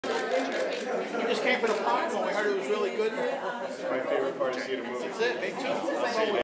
Jake Hoffman at A Glimpse Inside the Mind of Charles Swan...
Jake Hoffman at A Glimpse Inside the Mind of Charles Swan III - Arrivals at Landmark Sunshine Cinema on January 09, 2013 in New York, New York